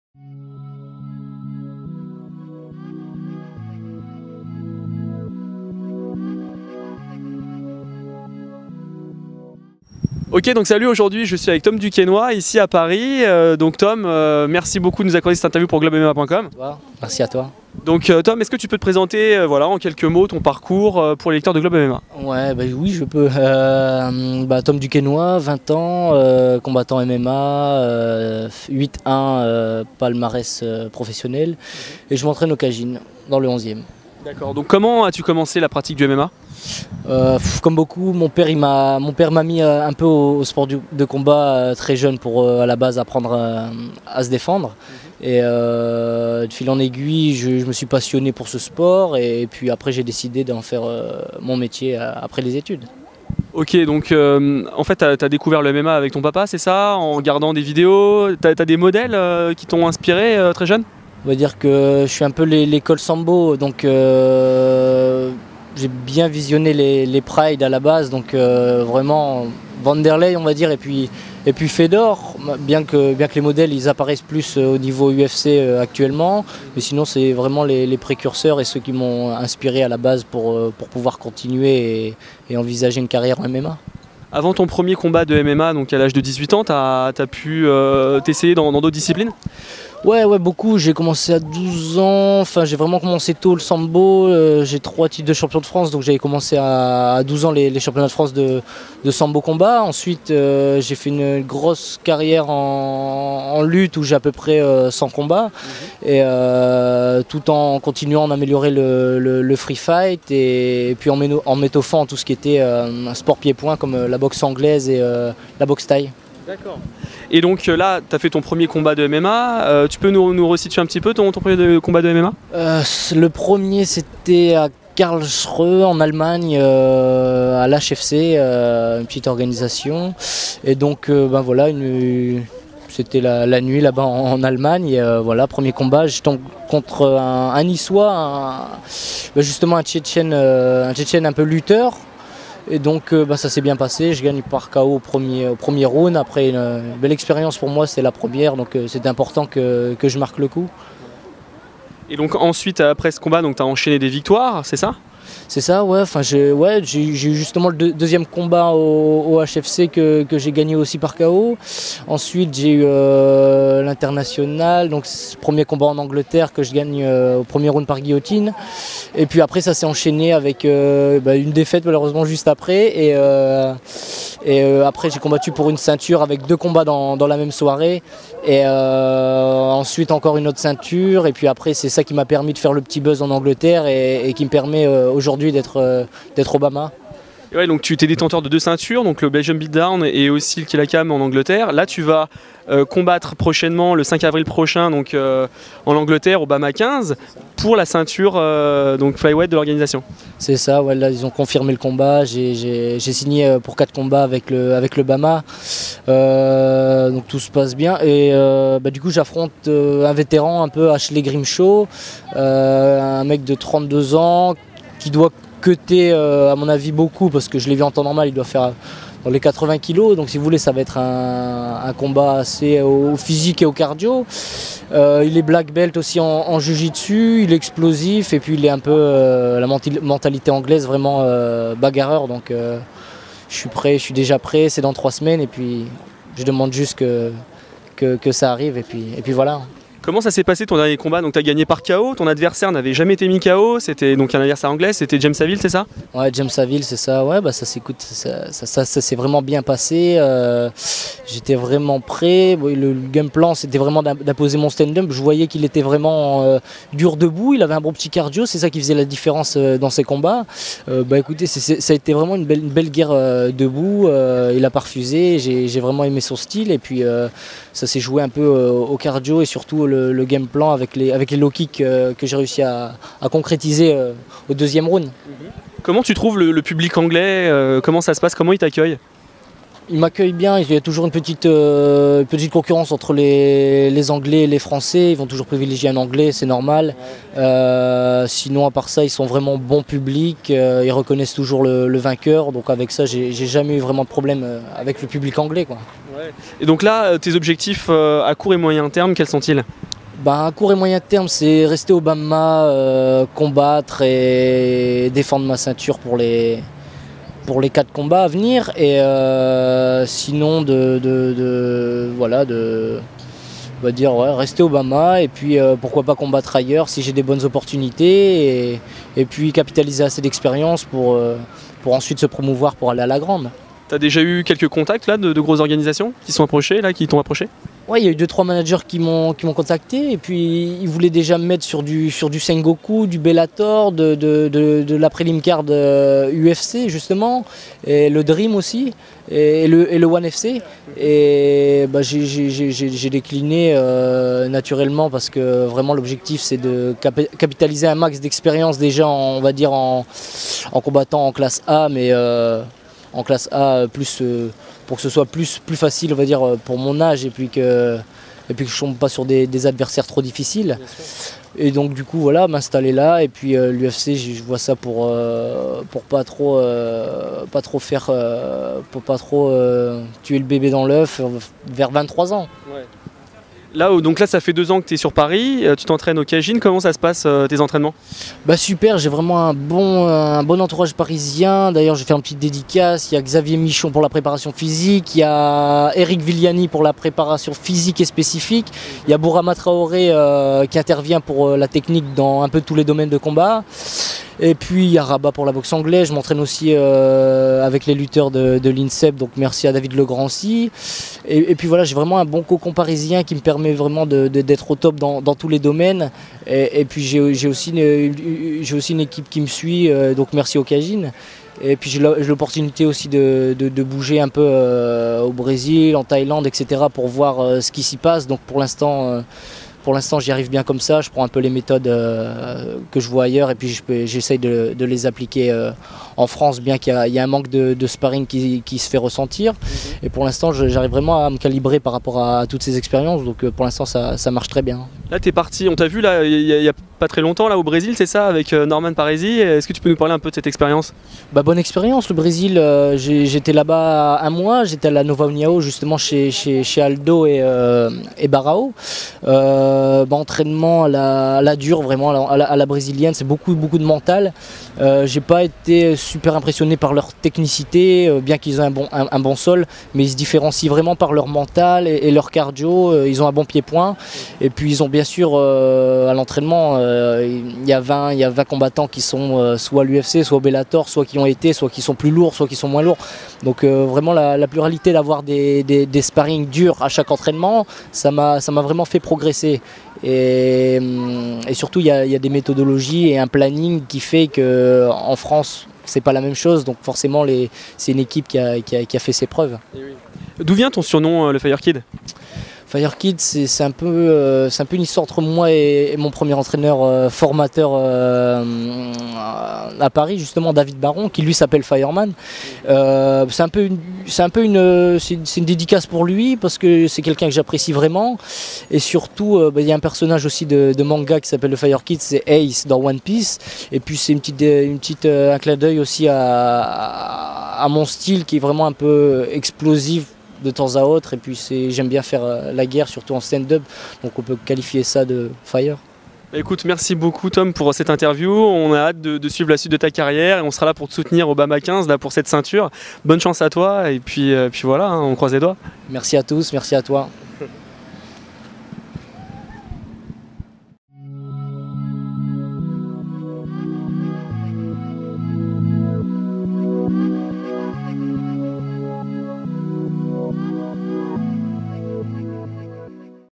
Interview de Tom Duquesnoy [BAMMA 15]
Voici une interview de Tom "Fire Kid" Duquesnoy, un jeune combattant français très prometteur qui combattra le 5 avril prochain pour la ceinture du BAMMA 15.